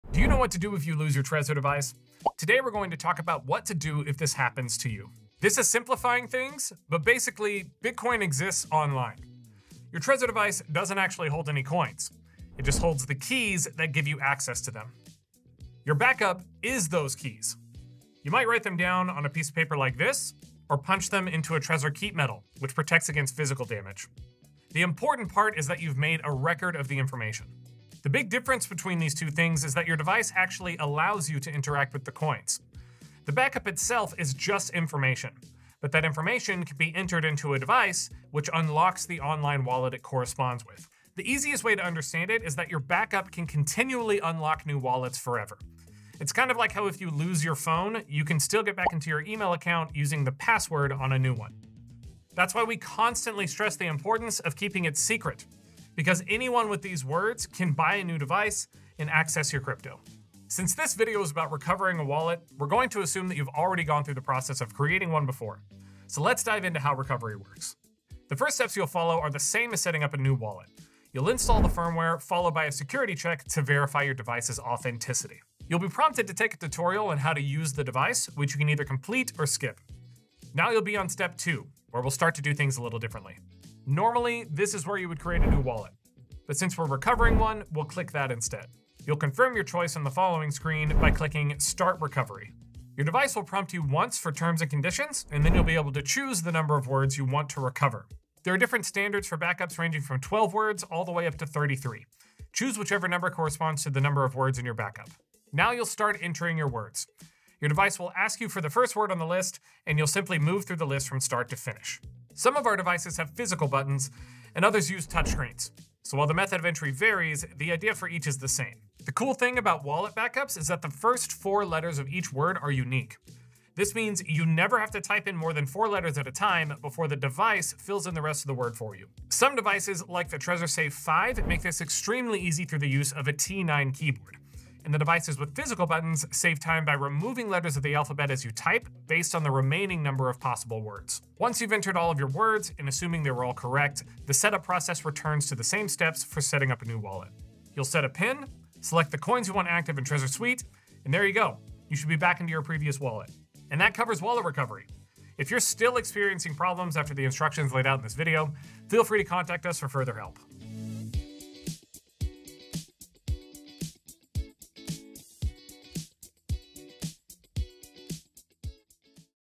How to Recover Your Wallet After Losing Your Trezor Device
Have you lost your Trezor device and need to recover your crypto wallet? Don’t worry — your funds are safe, and we’re here to guide you through the simple steps to get access back.

In this video, we’ll explain how Bitcoin is stored, the importance of your wallet backup, and provide a step-by-step tutorial on how to recover your wallet using your wallet backup.

Learn how to enter your wallet backup words, secure your device, and regain access to your crypto in Trezor Suite. Remember, your keys are your backup, so keep them safe and secure!